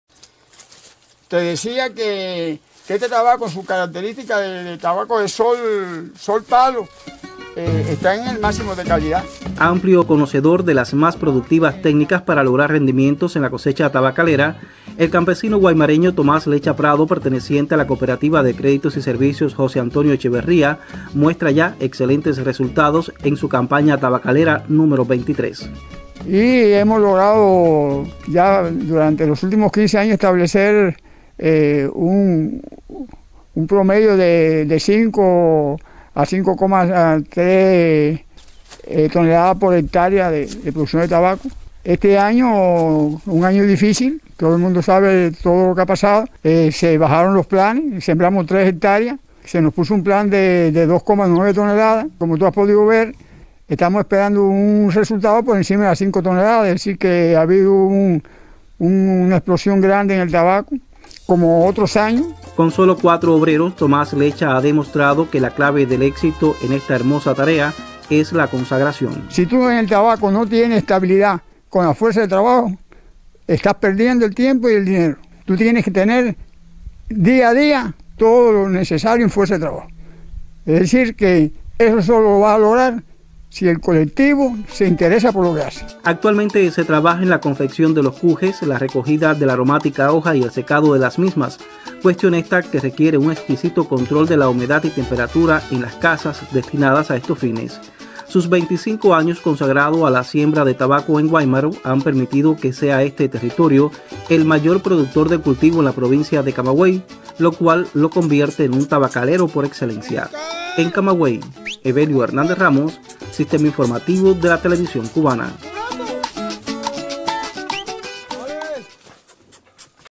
¿En qué condiciones está la producción de tabacos guaimareños?
En el municipio de Guáimaro se concentra más del 50% de la producción tabacalera de la presente campaña en la provincia de Camagüey, en la que el campesino Tomás Lecha Prado, perteneciente a la CCS José Antonio Echeverría de la comunidad rural de Palo Seco, tiene un alto compromiso.